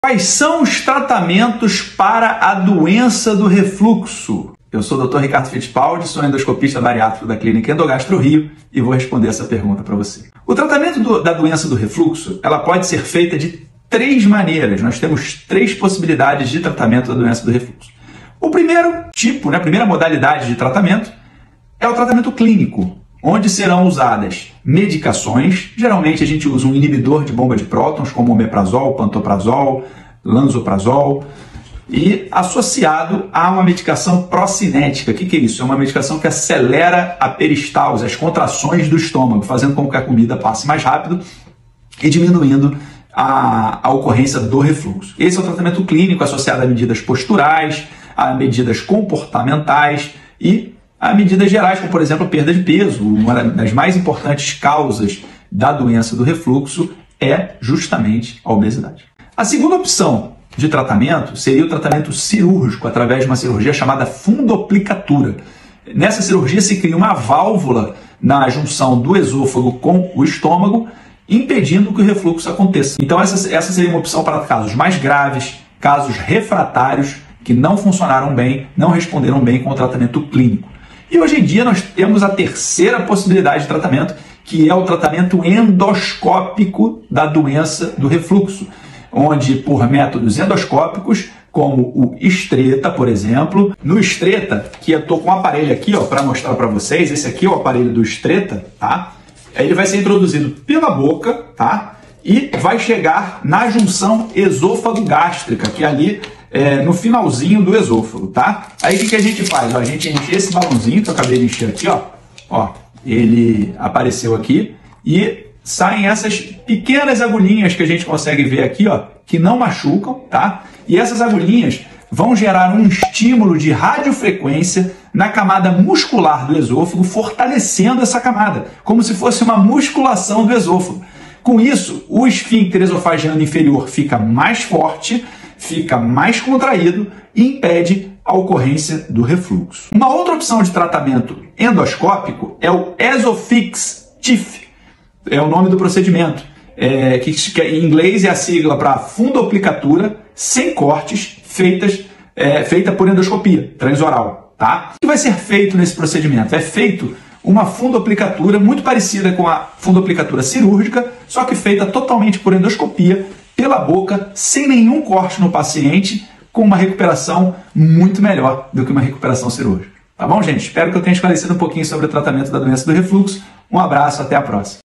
Tratamentos para refluxo
Você sofre com refluxo? Existem alguns tratamentos que podem te ajudar!

Assista o vídeo para entender como funciona o procedimento!